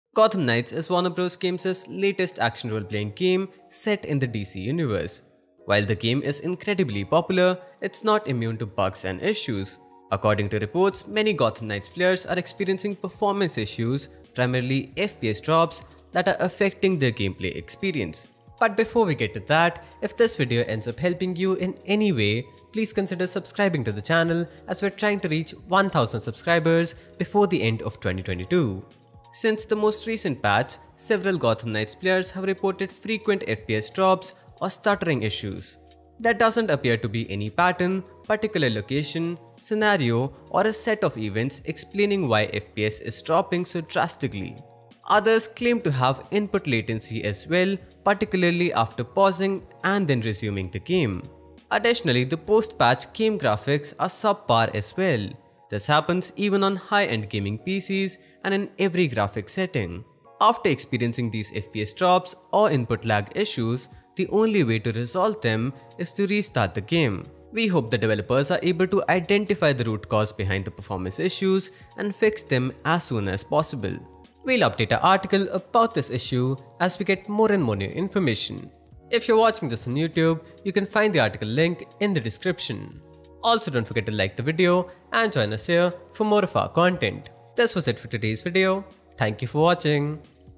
Gotham Knights FPS drops, stuttering or lower graphics quality after latest patch
Many Gotham Knights players report FPS drops or stuttering and lower graphics quality after the latest patch. Here's what we know.